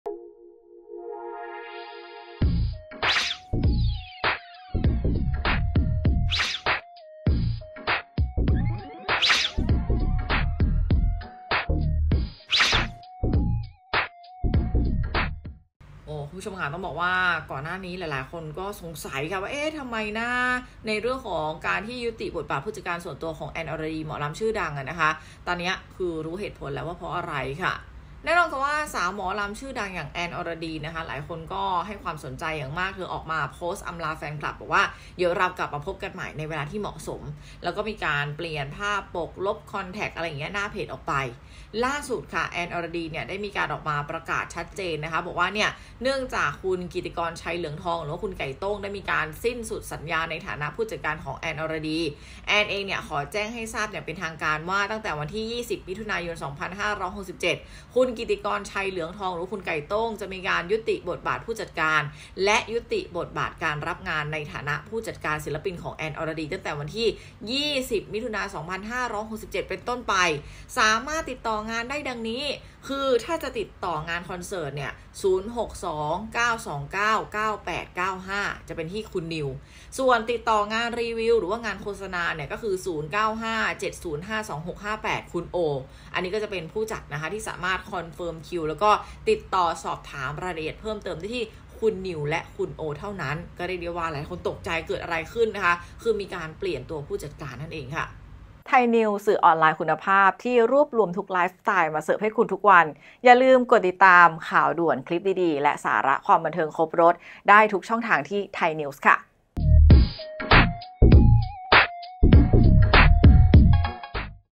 ’แอน อรดี‘ ยุติบทบาทผจก.ส่วนตัว แฟนคลับแห่ถาม ล่าสุดรู้สาเหตุแล้ว
ล่าสุดมีประเด็นที่ทำเอาหลายคนถึงกับสงสัยกันอีกแล้ว หลังแอนอรดี มีการยุติบทบาทผู้จัดการ และยุติบทบาทการรับงานของ ‘ไก่โต้ง’
ในฐานะผู้จัดการ ศิลปินของ แอน อรดี ล่าสุดรู้สาเหตุแล้ว